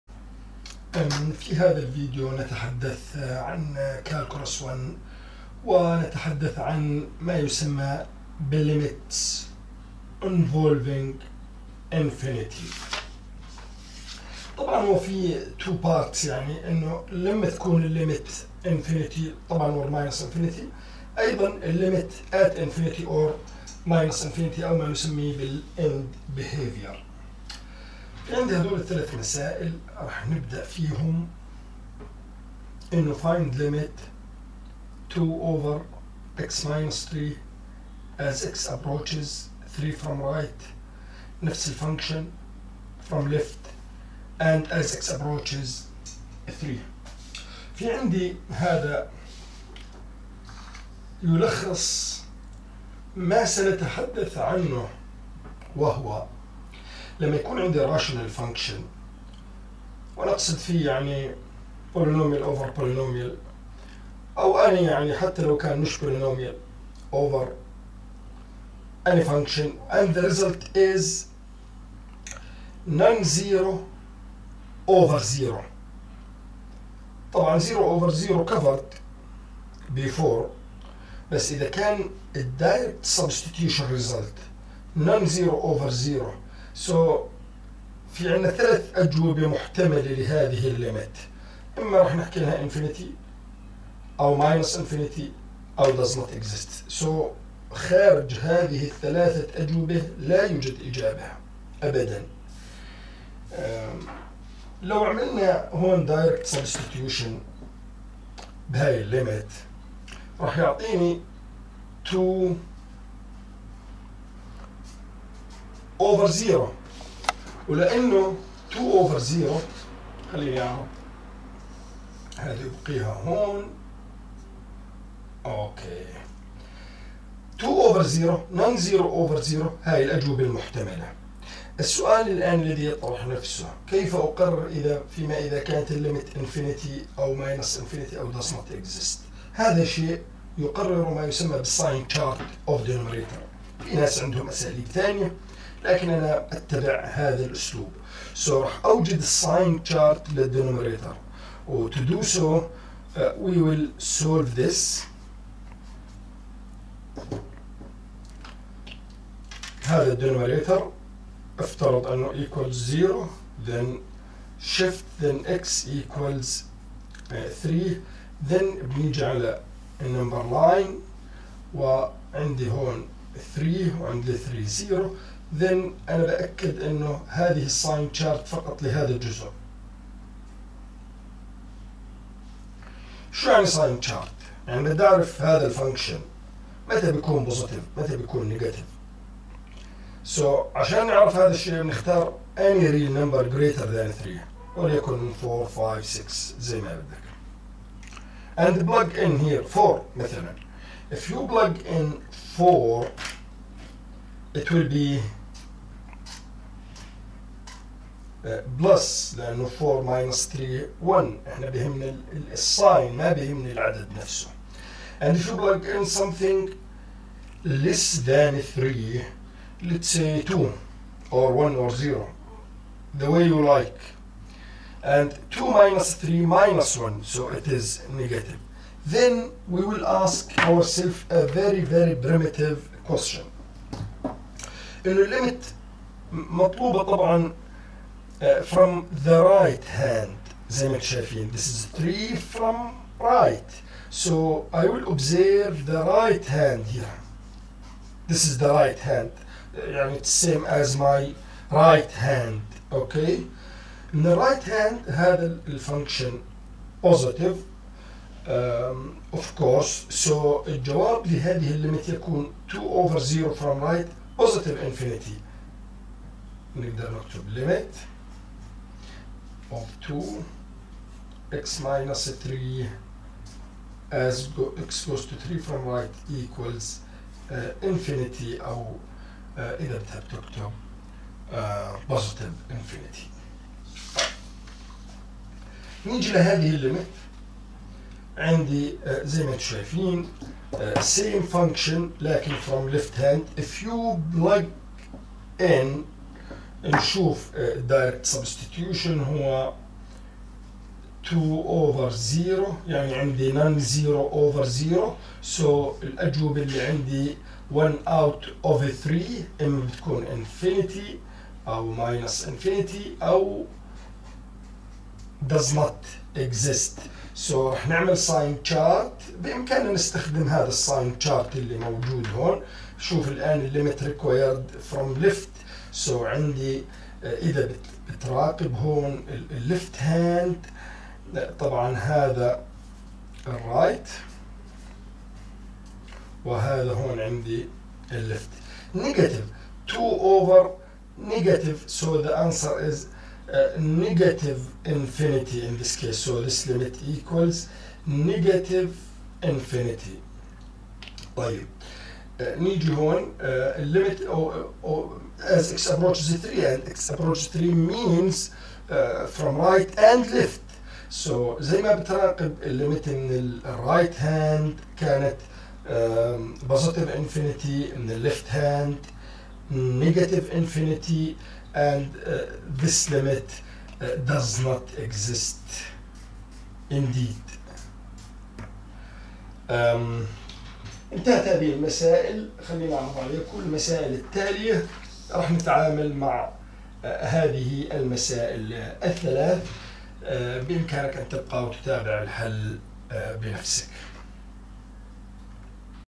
Calculus 1: Limits Involving Infinity-1
Calculus 1, Limits Involving Infinity,  for university students. The language of explanation is Arabic in addition to English when it is required.